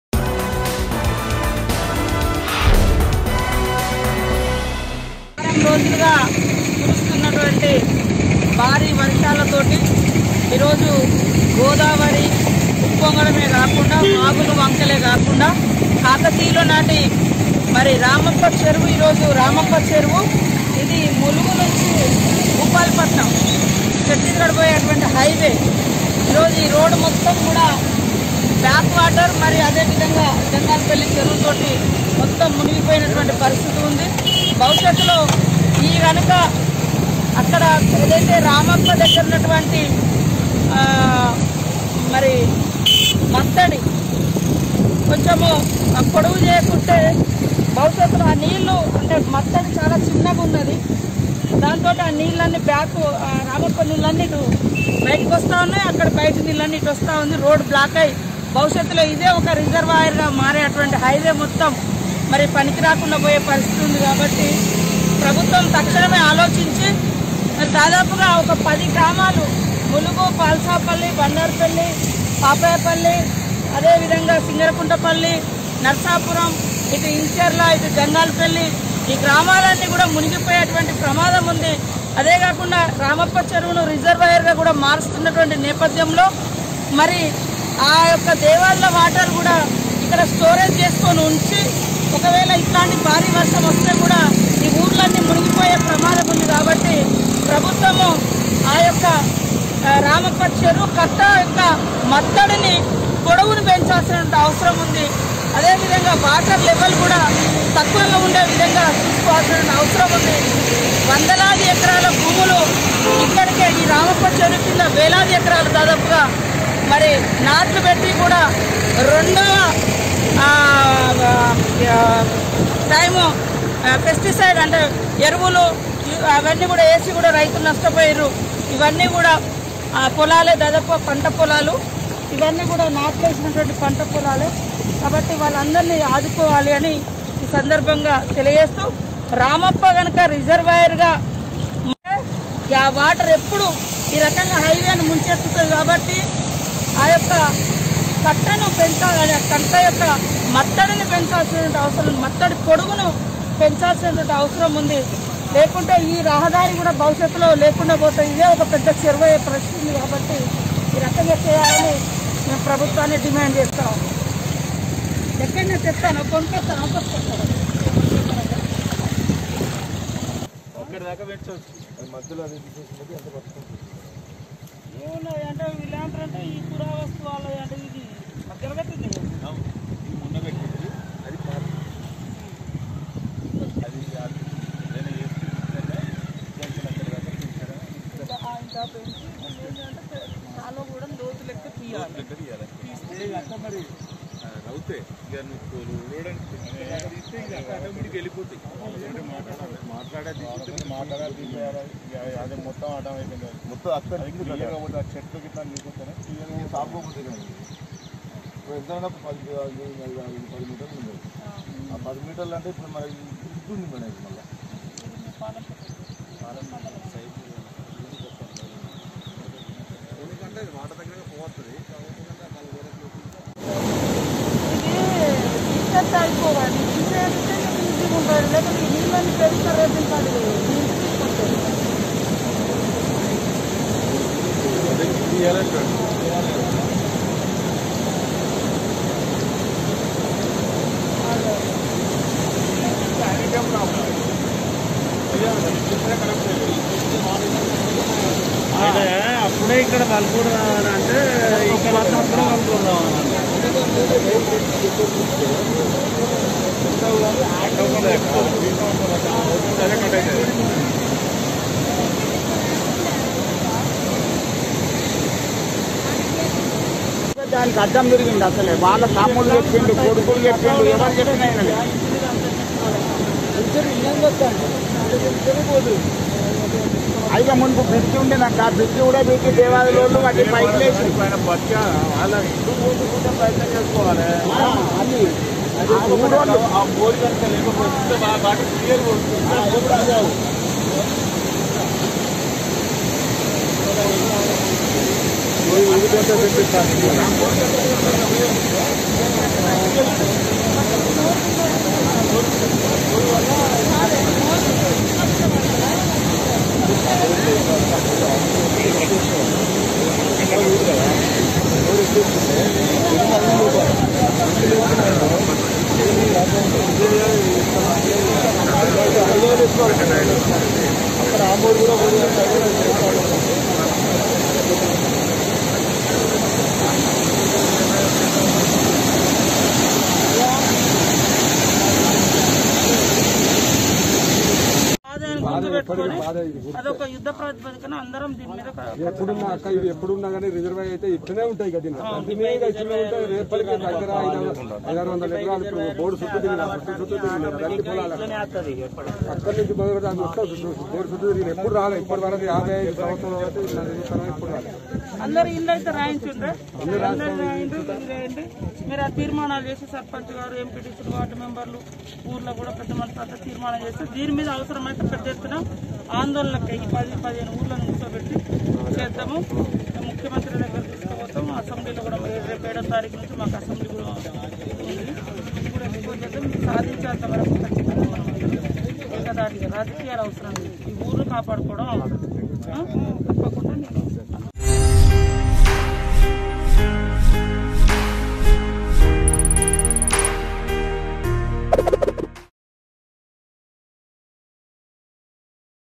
Congress MLA Seethakka Inspects Water Level On National Highway | Telangana Latest News | Mango News
Congress MLA Seethakka Inspects Water Level On National Highway.